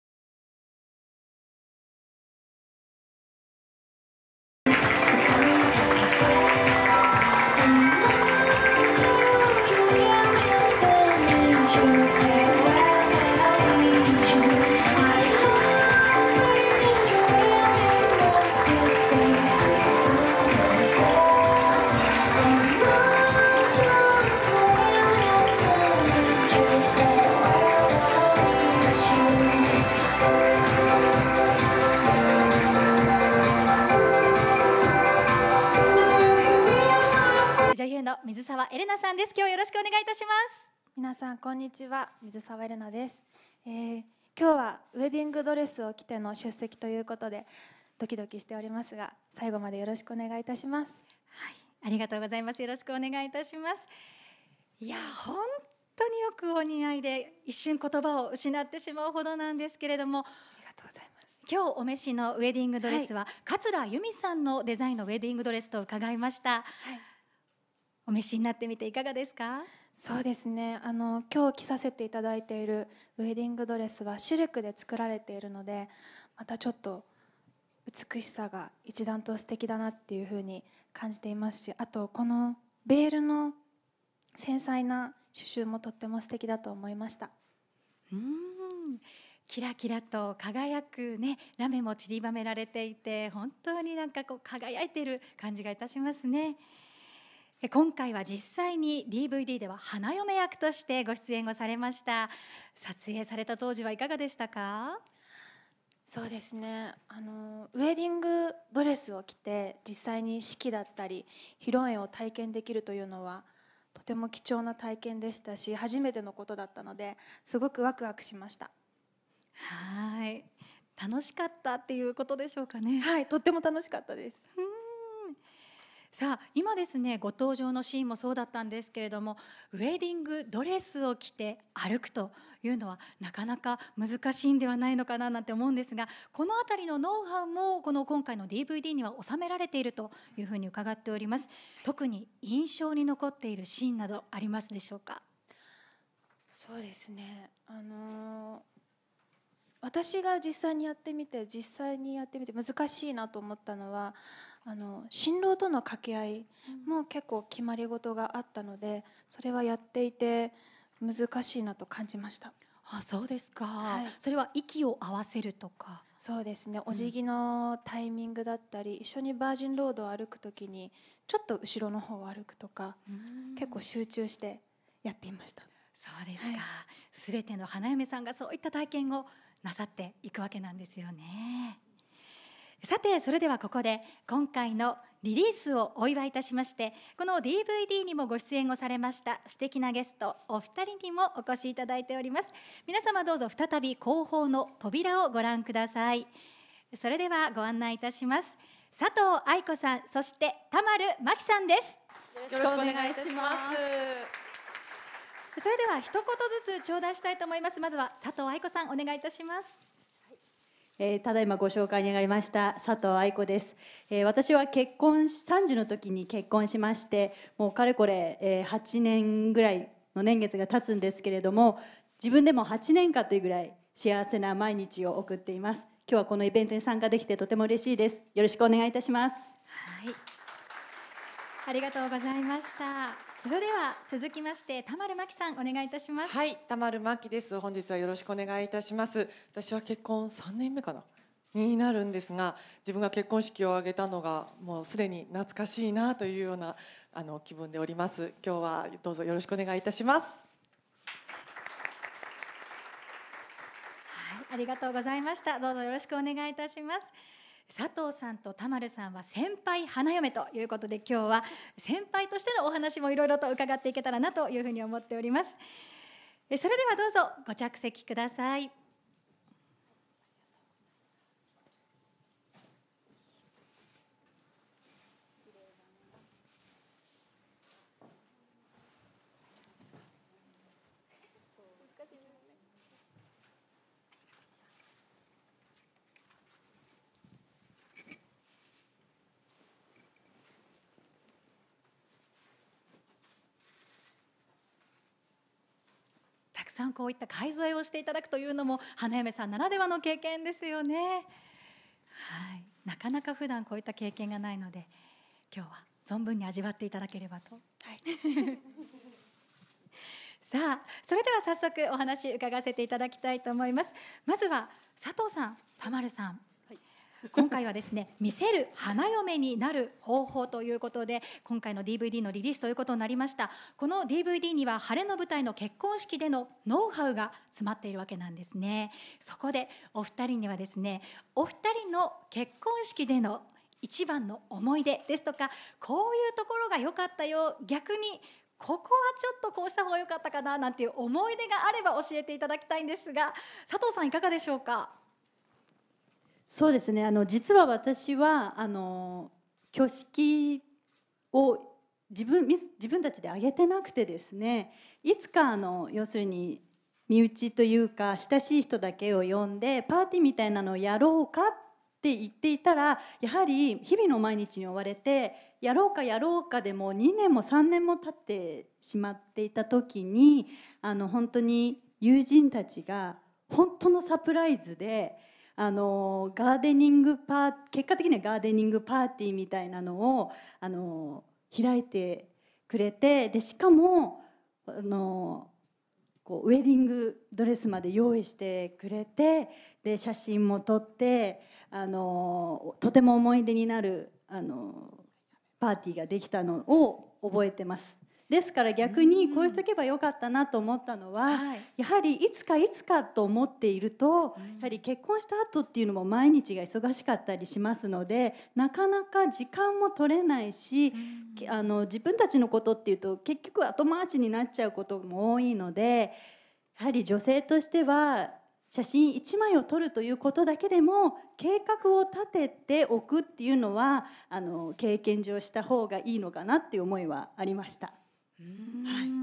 水沢エレナ、ウエディングドレス姿披露！先輩・田丸麻紀、佐藤藍子も登場 DVD「オスカープロモーション教育全集3『ウェディング編』」発売イベント１ Erena Mizusawa
女優の水沢エレナさんや田丸麻紀さん、佐藤藍子さんが東京都内でDVD「オスカープロモーション教育全集3『ウェディング編』～結婚式で“魅せる花嫁”になる方法～」の発表会見に登場。

　DVDは、水沢エレナさんら女優やモデルが数多く所属する芸能プロダクション「オスカープロモーション」が制作。結婚式を迎える花嫁が美しくいられるように、挙式での基本姿勢や歩き方、写真撮影時の笑顔や目線、立ち居振る舞いなどのテクニックを収録している。イベントには、同DVDに出演している水沢さんがウエディングドレス姿、先輩既婚女優として、田丸さんが黒のセクシーなロングドレス姿、佐藤さんが青いレースのドレス姿で登場した。DVDは12日発売、2800円（税抜き）。

Erena Mizusawa in a wedding dress! Senior Maki Tamaru, Aiko Satou also attended. DVD "Oscar Promotion's Education complete works 3 "Wedding Edition" Press Event 1

Actress Erena Mizusawa and Maki Tamaru, Aiko Saotu attended the press event for the DVD "Oscar Promotion's Education complete works 3 "Wedding edition" ~"How to become an attractive bride at the wedding ceremony" in Tokyo.

The DVD is produced by arts production OSCAR PROMOTION that represents many actresses and models such as Erena Mizusawa.